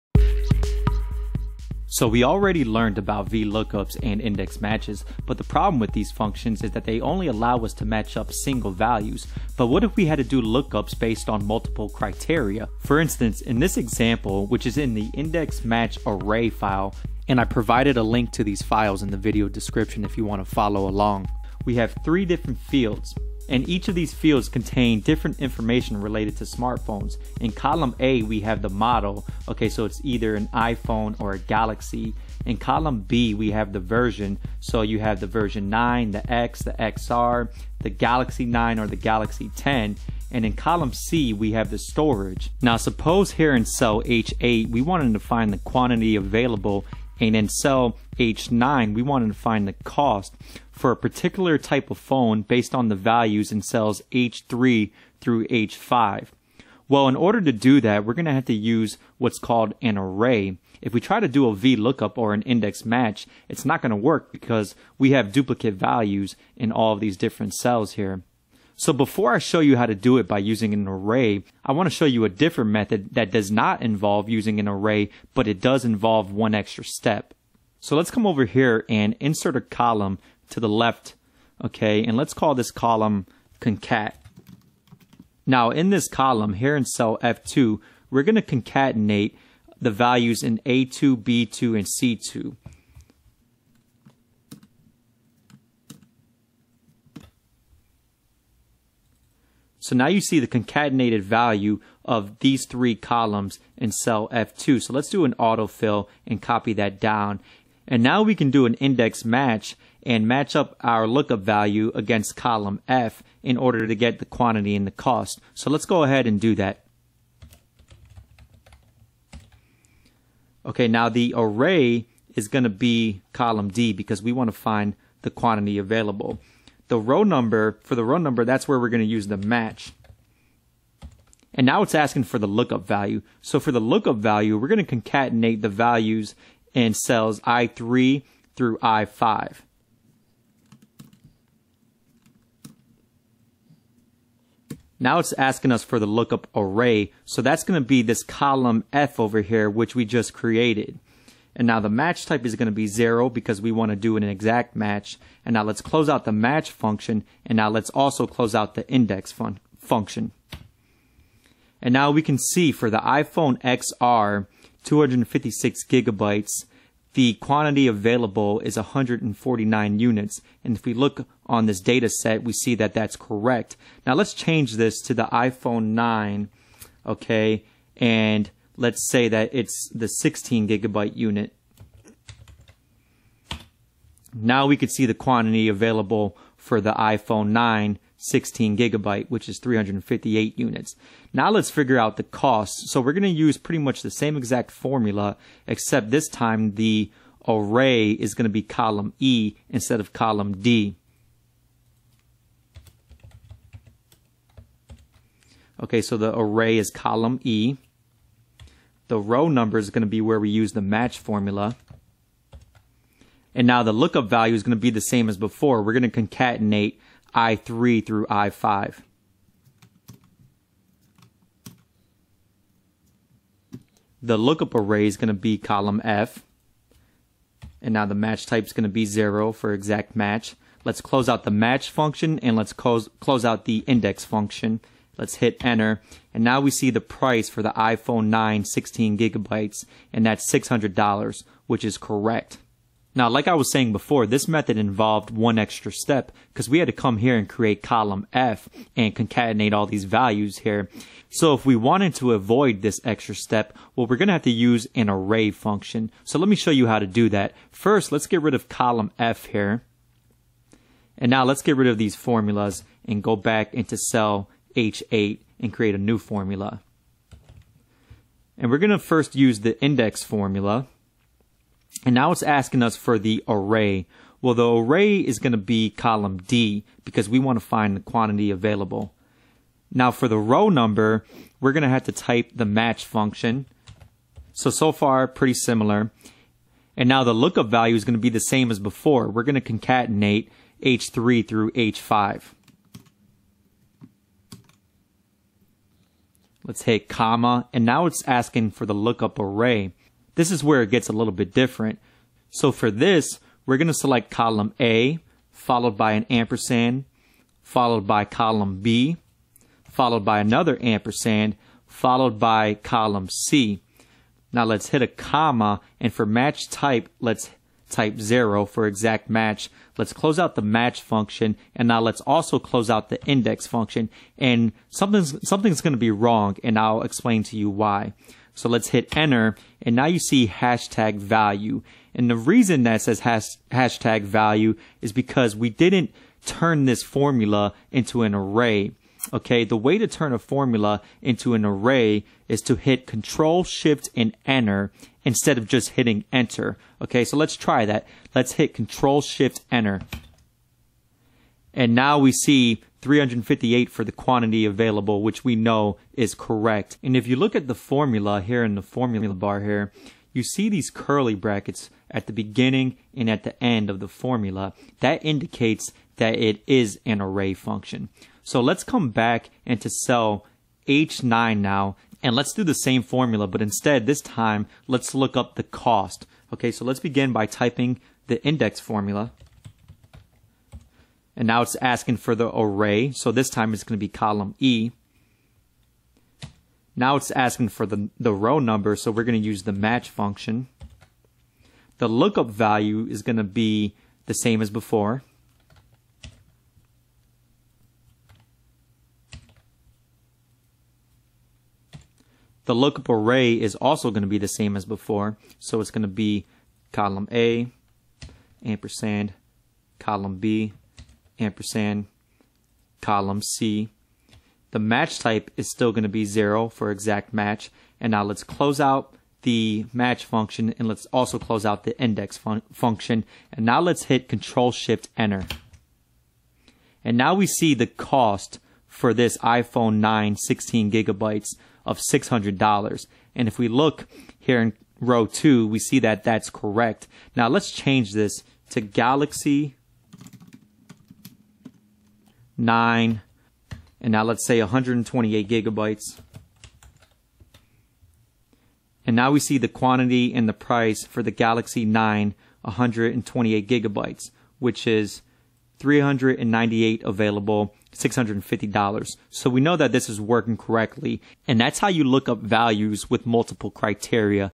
Index Match with Multiple Criteria
This video explains how to perform a lookup for a value based on multiple criteria. A normal vlookup or index match will not work since you only provide one criteria for these types of lookups. Instead, we have to use an array. Below is the link to the file that I use in this video: